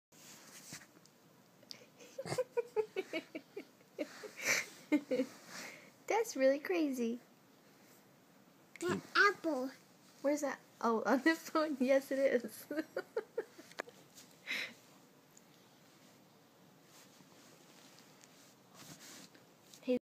Zeia Teeth Wiggle
Zeia showing us her new trick.